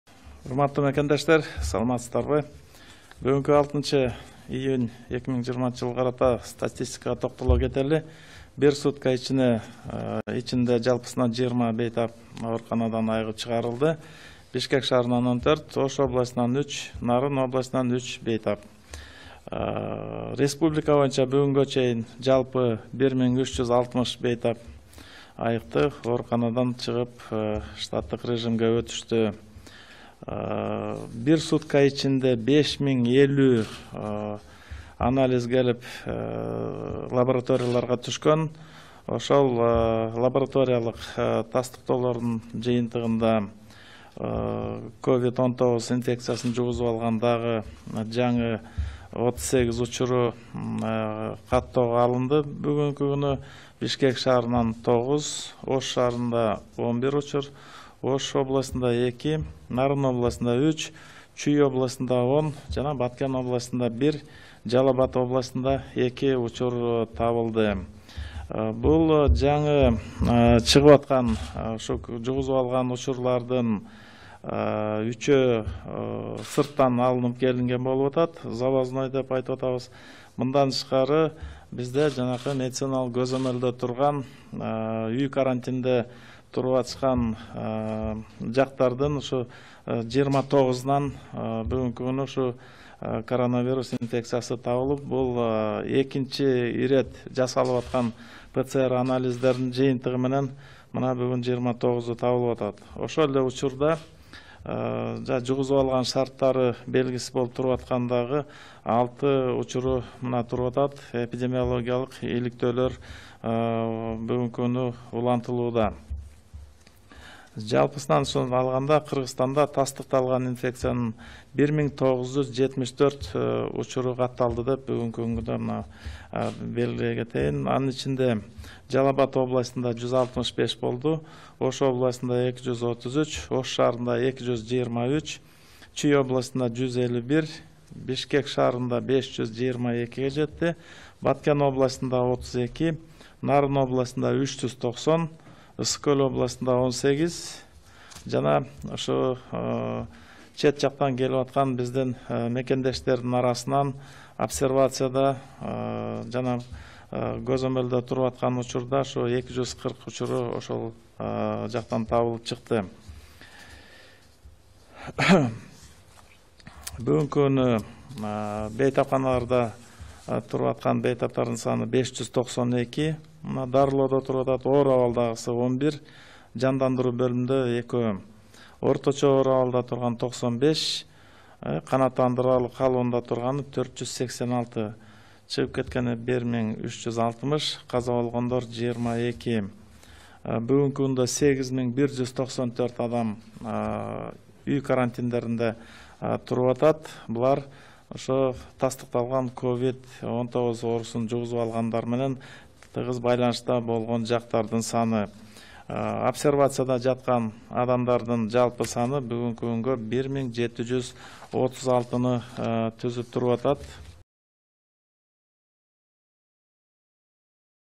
Кыргызстан: 6-июнда коронавирус 38 кишиден табылды
Кыргызстанда соңку бир сутка ичинде коронавирус жуккан 38 адам жаңы катталды. Бул тууралуу 6-июнда саламаттык сактоо министринин орун басары Нурболот Үсөнбаев билдирди.

Бир сутка ичинде 20 бейтап айыгып чыкты.

Инфекция жуккандардын саны: Бишкек шаарында – 9, Чүй облусу – 10, Нарын облусу – 3, Ош шаары – 11, Ош облусу – 2, Баткен облусу – 1, Жалал-Абад облусу – 2 киши.

Жалпысынан 6-июнга карата Кыргызстанда коронавирус 1974 адамдан табылды, алардын ичинен 1360 бейтап сакайып кетти. COVID-19 вирусу табылган 22 бейтап кайтыш болгон. COVID-19 илдети аныкталгандар менен байланышта болгондорсун саны 8 194 киши. Обсервацияда 1 736 адам жатат.